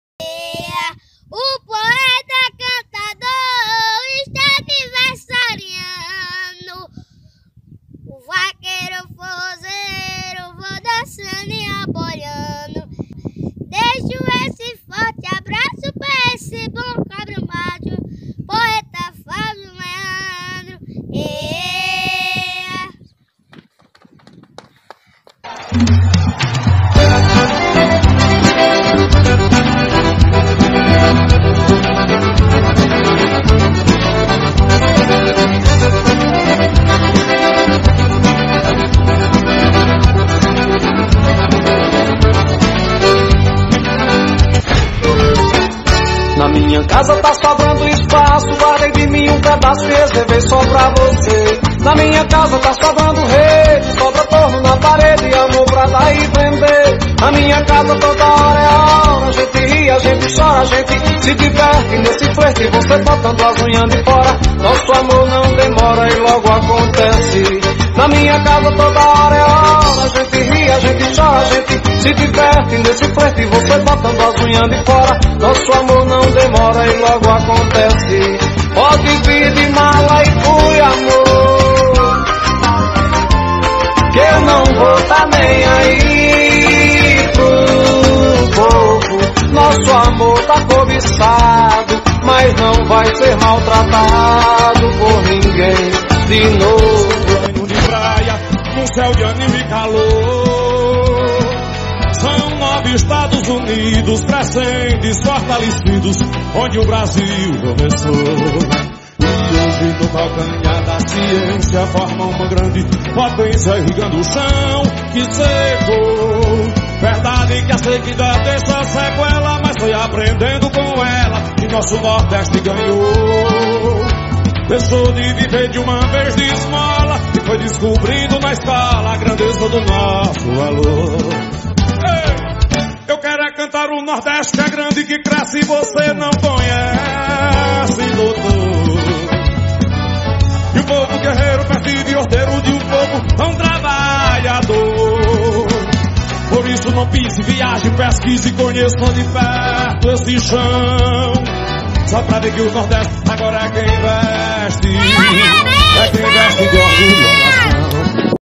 HOMENAGEM A FLÁVIO LEANDRO
Vaqueiro Forrrozeiro e Lara Selena, os forrozeiros mirins mais arrochados do Brasil, parabenizando o Poeta Cantador pela passagem de seu aniversário natalício.
São 52 anos de muita poesia e alegria nos palcos do nosso país.

Sigam o casalzinho no Instagram...